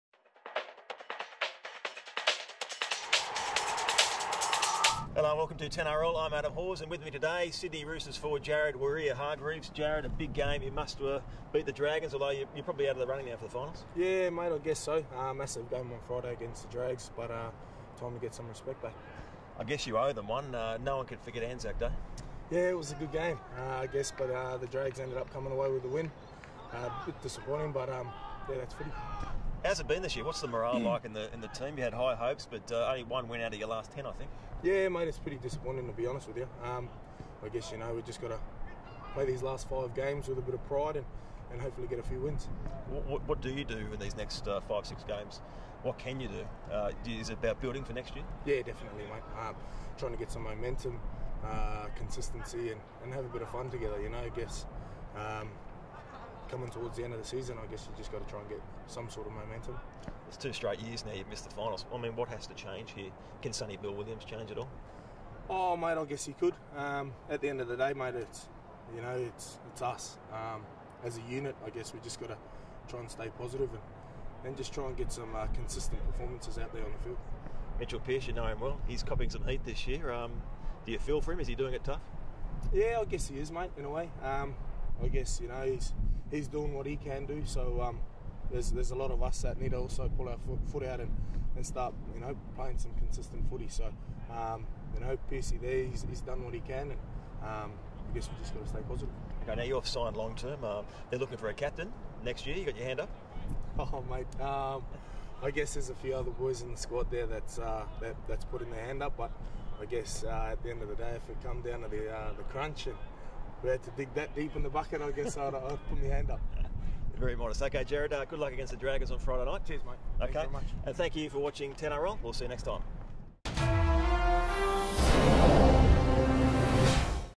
TeNRL
Hawsie speaks with Sydney Roosters prop Jared Waera-Hargreaves on the approaching end of season and half-back Mitchell Pearce